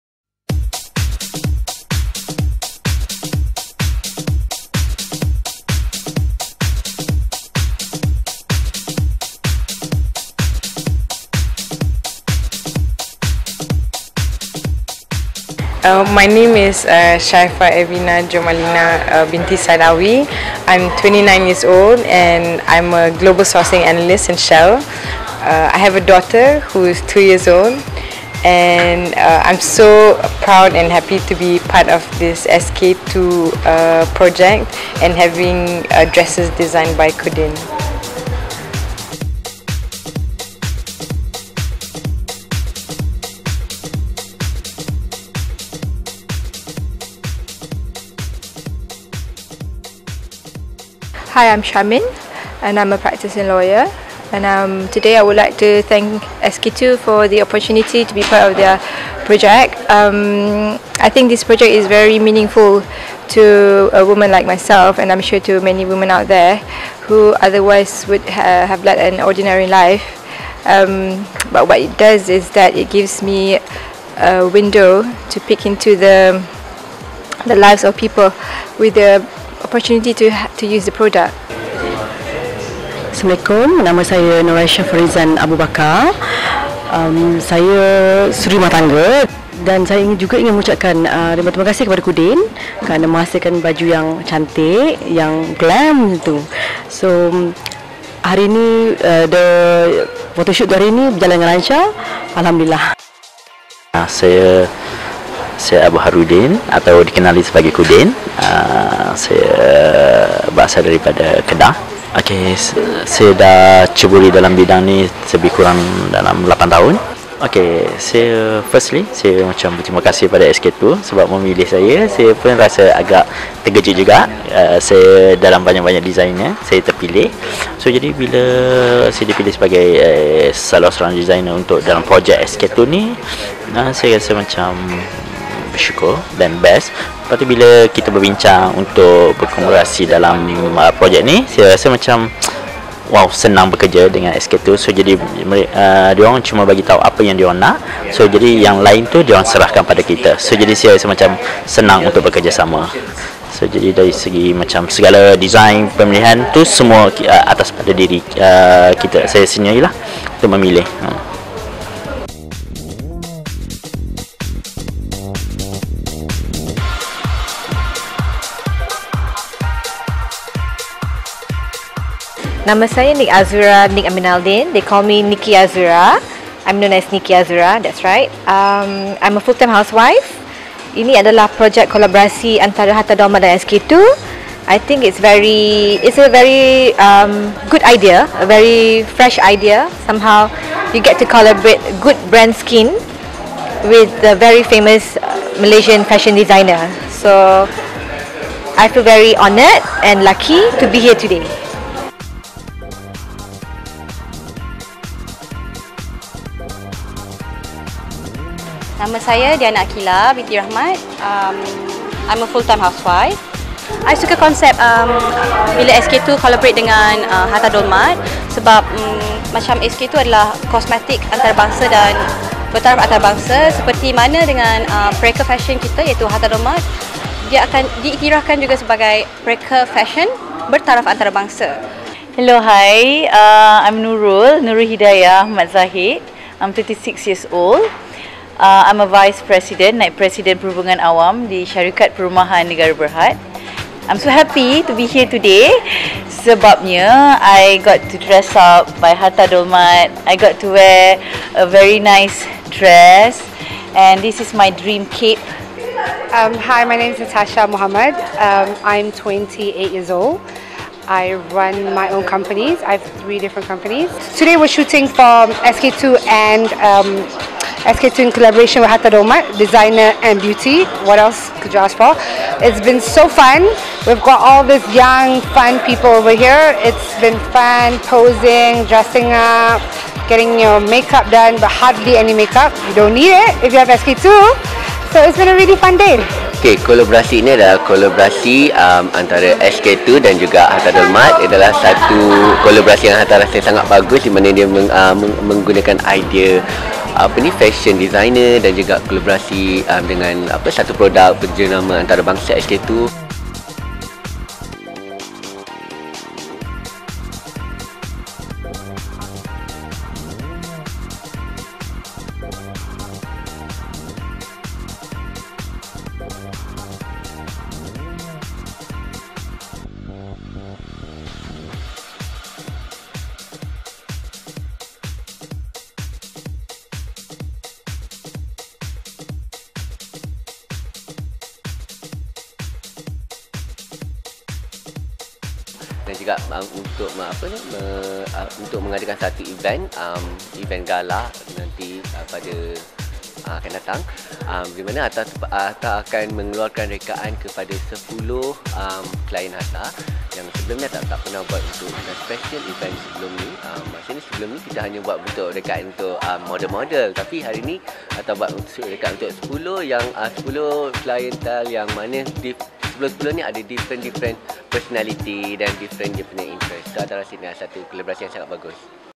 Behind The Scene - Pesona Unggul SK-II
Ikuti di sebalik tabir sesi fotografi eksklusif GLAM bersama antara barisan peserta yang telah melalui transformasi bersama SK-II dan juga menyarung busana indah rekaan Hatta Dolmat dan Qudyn.